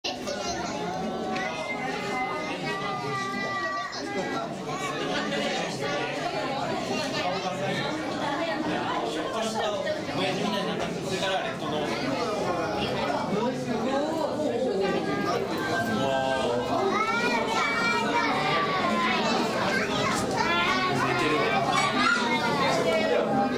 淡水魚水族館の栃木県なかがわ水遊園へ行って来ました～！ 2016年8月15日 00327
淡水魚水族館の栃木県なかがわ水遊園へ行って来ました～！
2016年8月15日